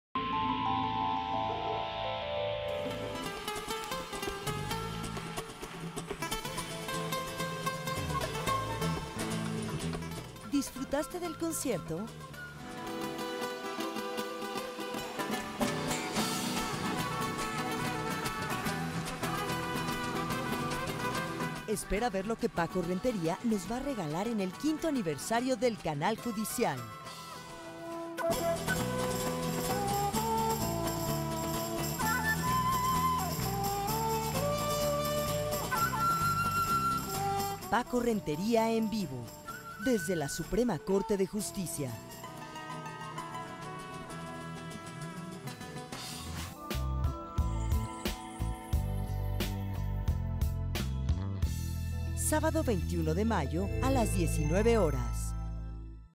Paco Renteria EN VIVO / Concierto del 5to Aniversario del Canal Judicial
Paco Renteria en vivo desde la Suprema Corte de Justicia de la Nación.

Sábado 21 de Mayo en punto de las 19:00 hrs.

Concierto del 5to Aniversario del Canal Judicial.